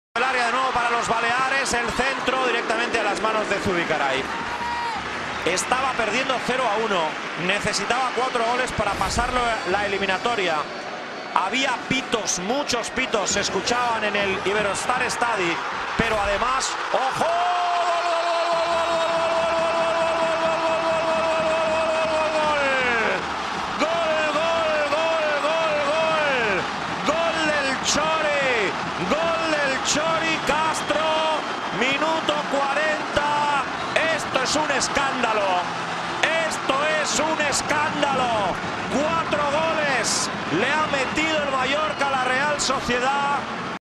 Biggest Goalkeeper Fail Ever
Real Sociedad keeper Enaut Zubikarai made the funniest and best goalkeeper error ever!!!